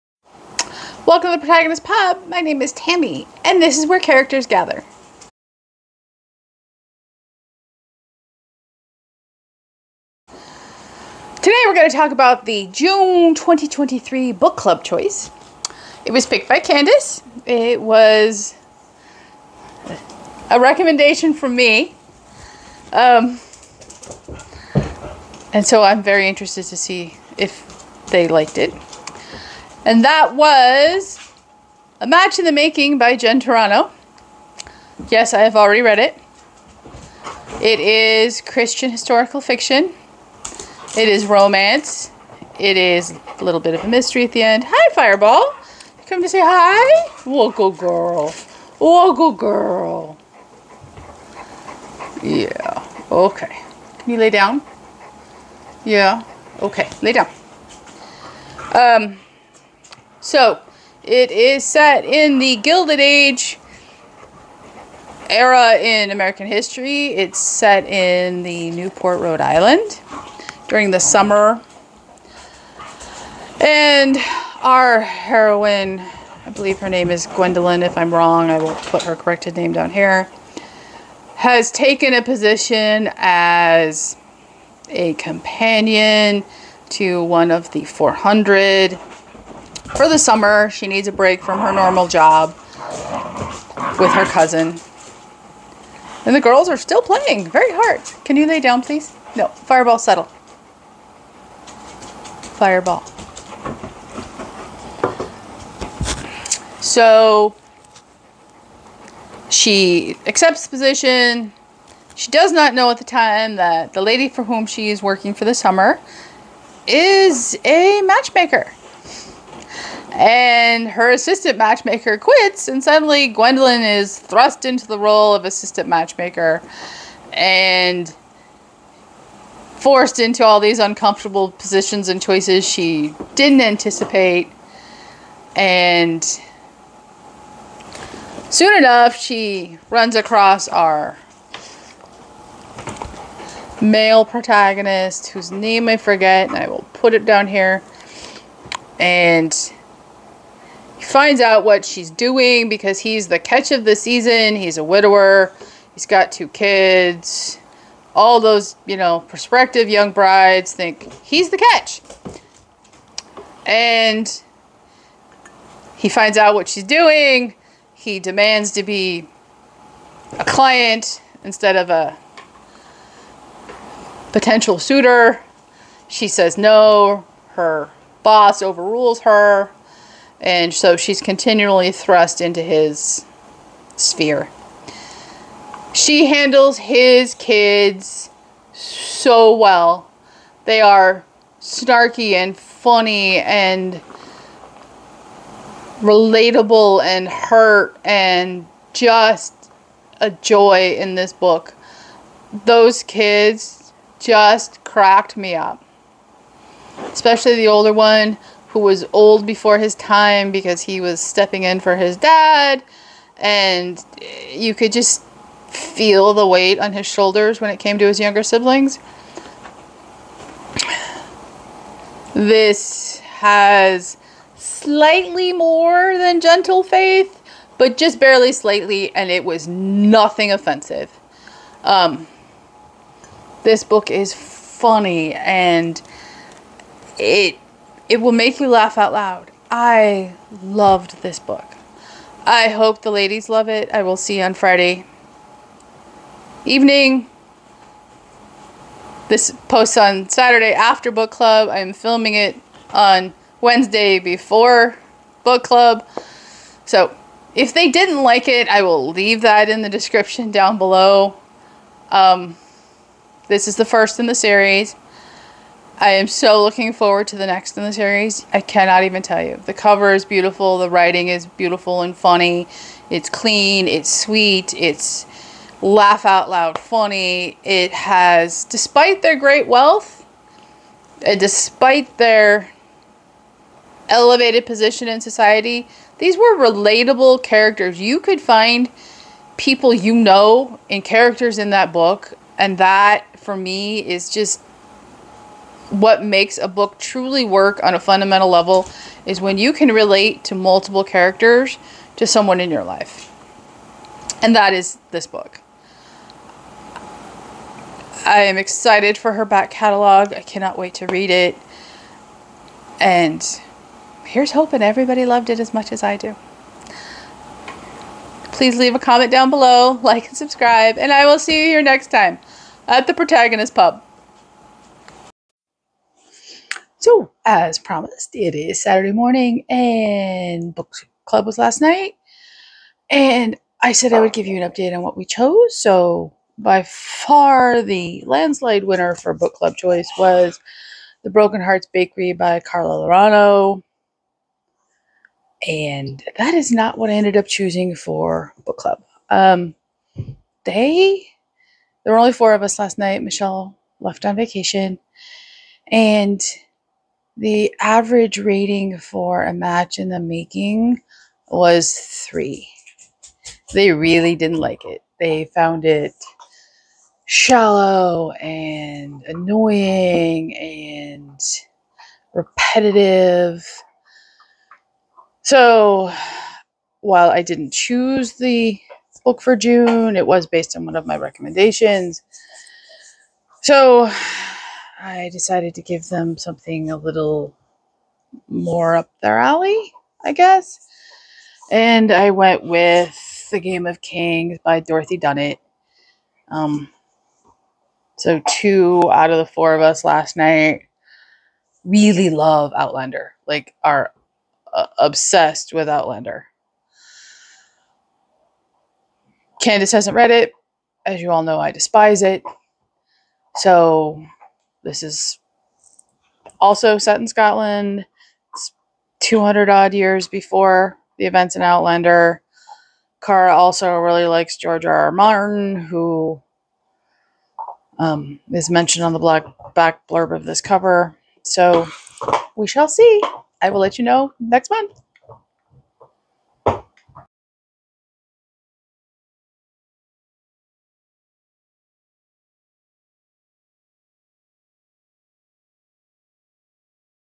June 2023 Book Club
The review of the June 2023 Book Club choice and the announcement of the book I picked for July.